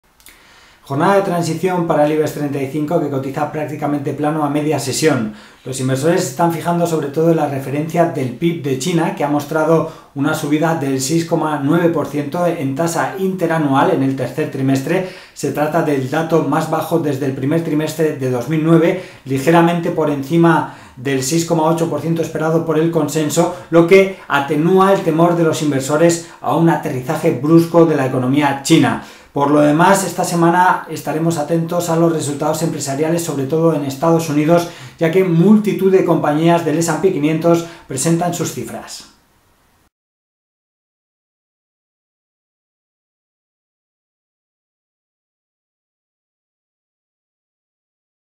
El Ibex consolida en 10.200 puntos
El Ibex consolida por encima de 10.200 puntos a media sesión, en una jornada tranquila para la renta variable europea y en dónde nos preguntamos cuáles son las claves para invertir con éxito.

La actualidad de la jornada se centra en los datos económicos publicados en China, que han mostrado una desaceleración del crecimiento, pero no un colapso económico, como habían descontado algunos inversores.